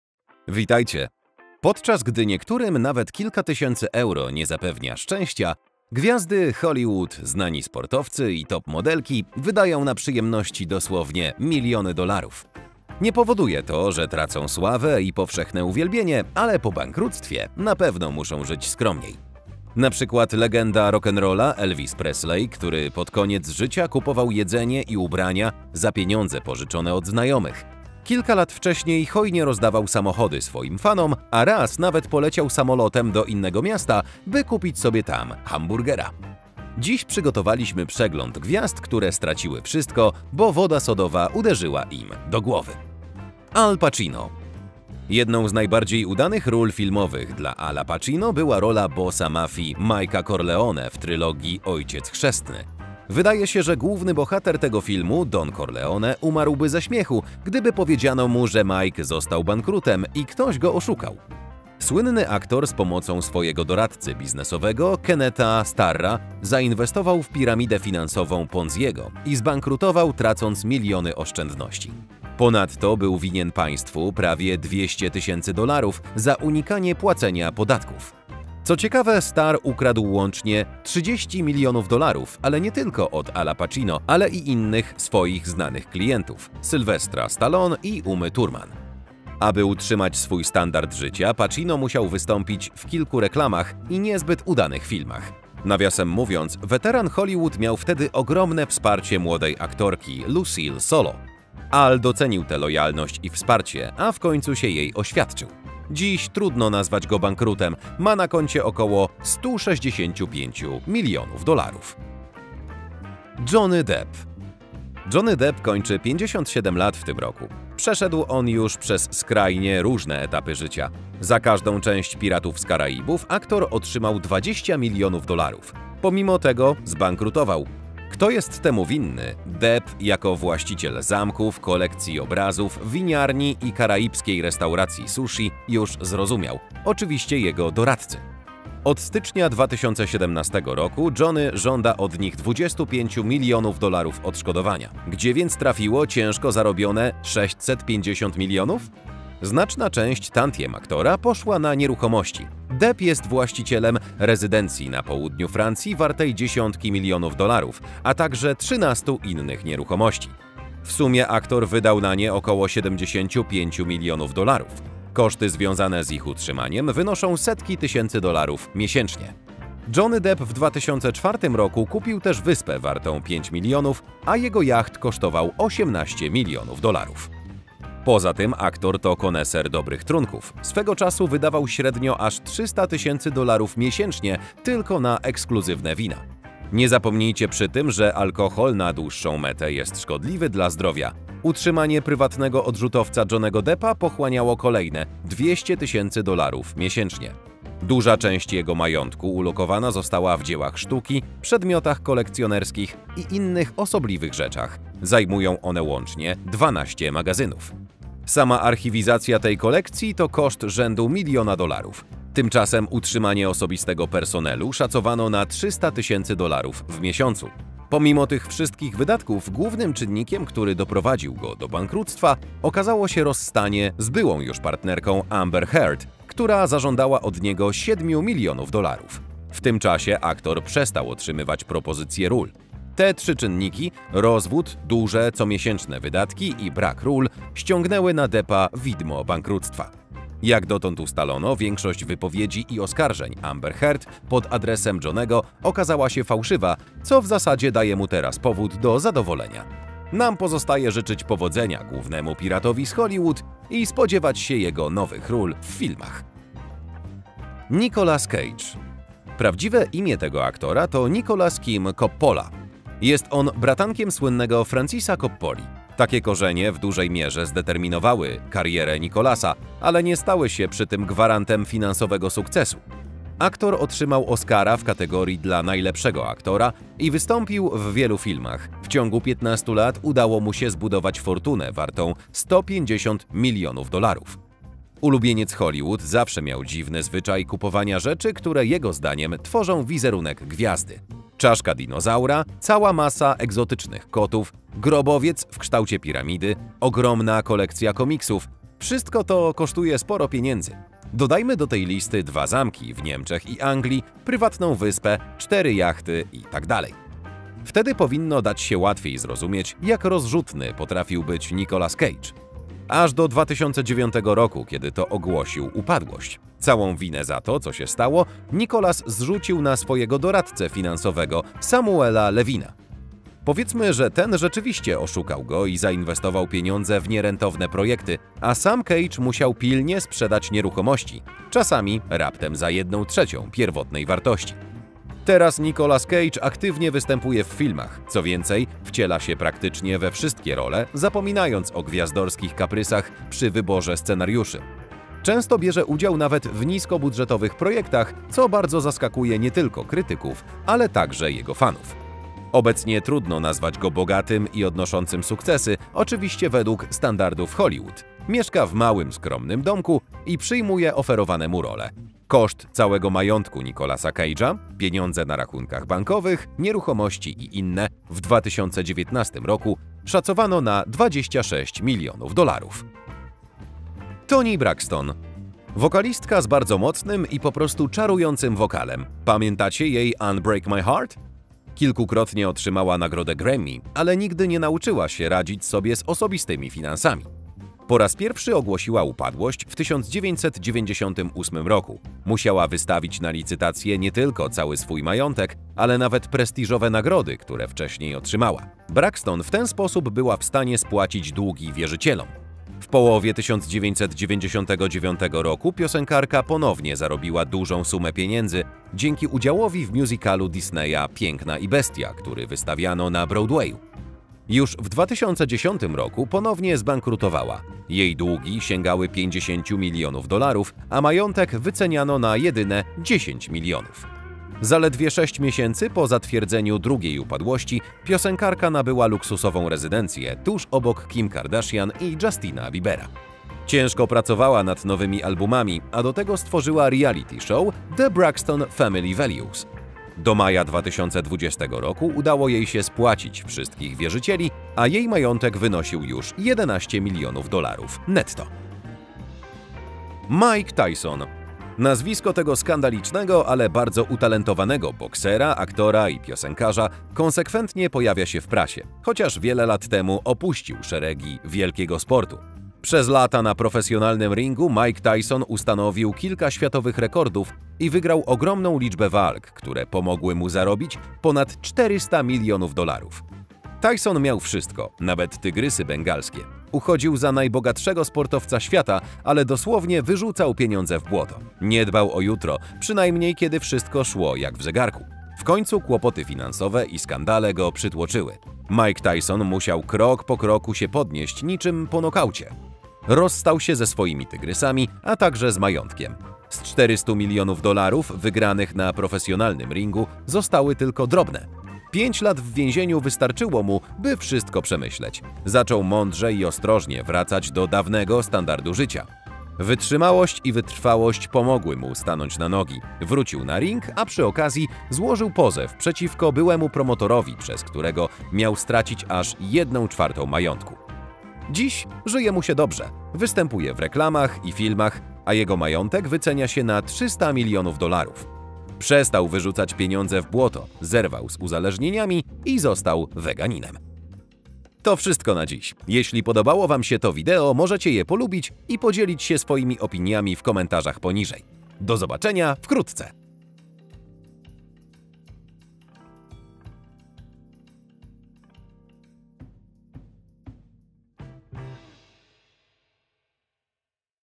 5 Gwiazd, Które Straciły Wszystko, Bo Woda Sodowa Uderzyła Im Do Głowy!
Podczas gdy niektórym nawet kilka tysięcy euro nie zapewnia szczęścae, gwiazdy Hollywood, znani sportowcy i top modelki wydają na przyjemności dosłownie miliony dolarów. Nie powoduje to, że tracą sławę i powszechne uwielbienie, ale po bankructwie na pewno muszą żyć skromniej. Na przykład, jak legenda rock and rolla Elvis Presley, który pod koniec życia kupował jedzenie i ubrania za pieniądze pożyczone od znajomych. Kilka lat wcześniej hojnie rozdawał samochody swoim fanom, a raz nawet poleciał samolotem do innego miasta, by kupić sobie tam… hamburgera. Dziś przygotowaliśmy przegląd gwiazd, które straciły wszystko, bo woda sodowa uderzyła im do głowy.